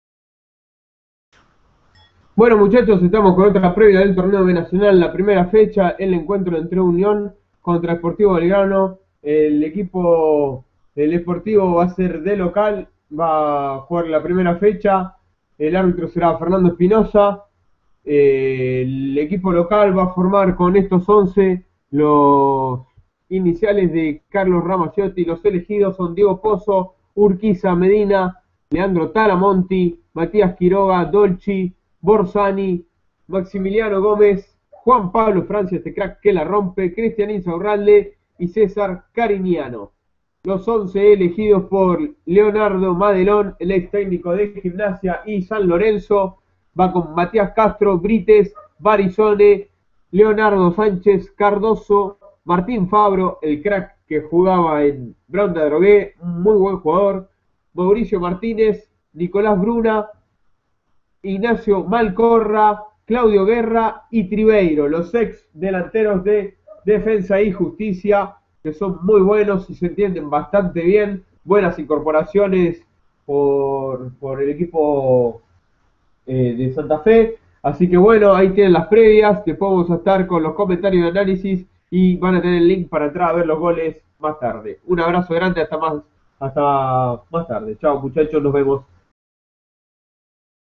Torneo B Nacional:Sportivo Belgrano Vs Union - Fecha 1 - Previa del partido - Formaciones
SP. BELGRANO : Diego Pozo, Emanuel Urquiza, César Medina, Leonardo Talamonti, Matías Quiroga, Franco Dolci, Leonardo Borzani, Maximiliano Gómez, Juan Pablo Francia, Cristian Insaurralde, César Carignano. DT: Carlos Ramacciotti.

UNION: Matías Castro, Emanuel Brítez, Diego Barisone, Leonardo Sánchez, Marcelo Cardozo, Martín Fabro, Mauricio Martínez, Nicolás Bruna, Ignacio Malcorra, Claudio Guerra, Enrique Triverio. DT: Leonardo Madelón.

ARBITRO: Fernando Espinoza

CANCHA: SP. BELGRANO (SAN FRANCISCO)